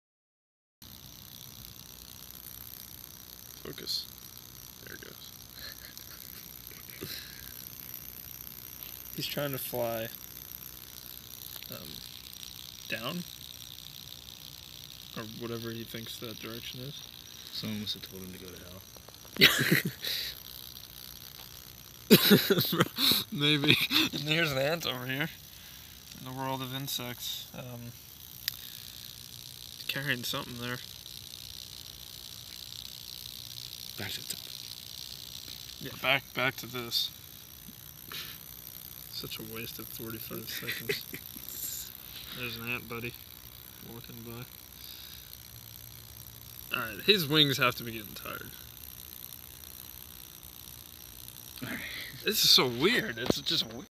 Beetle flying into the ground
A beetle flying into the ground, it went on for like 20 minutes... I think it's wing was damaged or something.